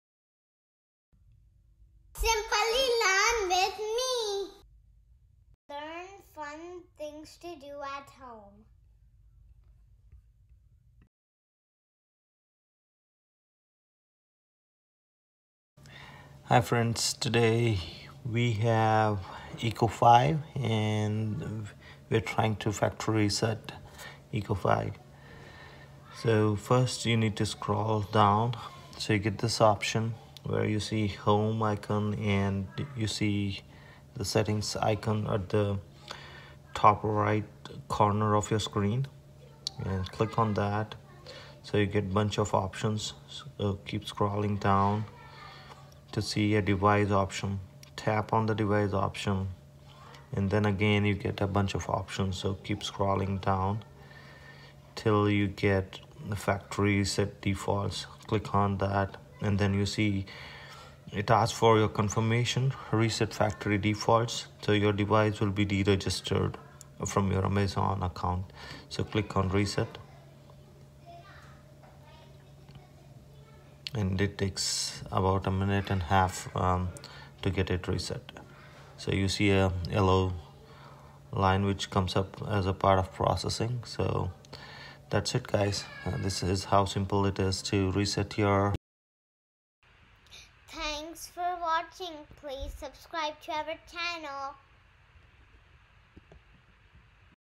echo show 5 factory reset in simple steps | how to reset echo show factory reset
This video from simply learn with me will show how easily we can reset the echo show 5 to factory reset.